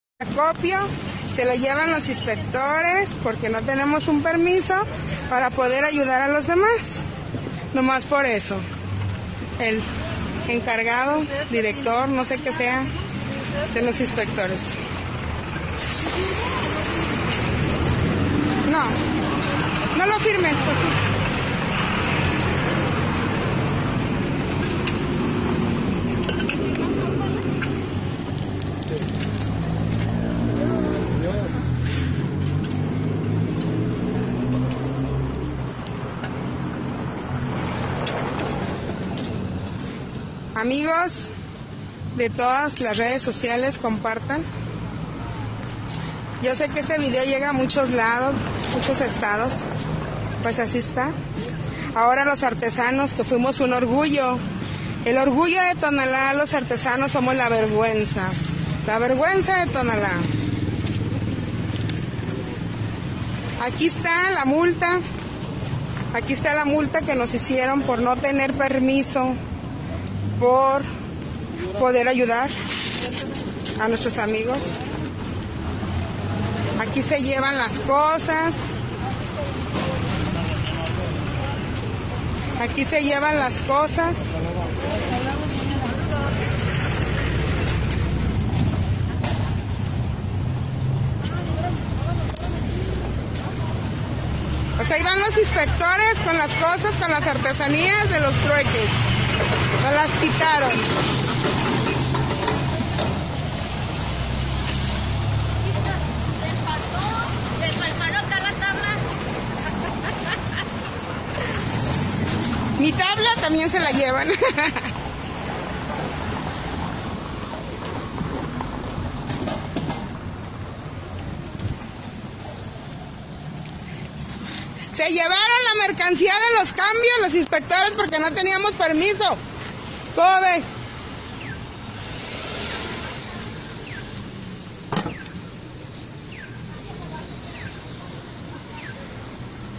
ARTESANOS DE JALISCO INTENTAN CAMBIAR SU PRODUCTO POR COMIDA Y SON MULTADOS
Ese fascista y represor de Enrique Alfaro está llegando a niveles insospechados de crueldad… Unos artesanos de Jalisco que intentaban intercambiar su producto por comida fueron amedrentados por los inspectores del gobernador y después multados… A ver si así se ponen con los narcos